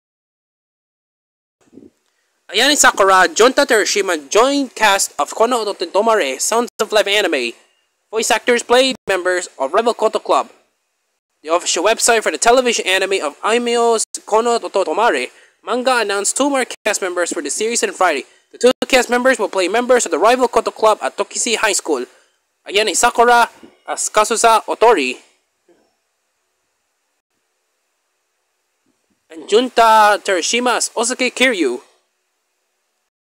Ayane Sakura, Junta Terashima Join Cast of Kono Oto Tomare!: Sounds of Life Anime
Voice actors play members of rival koto club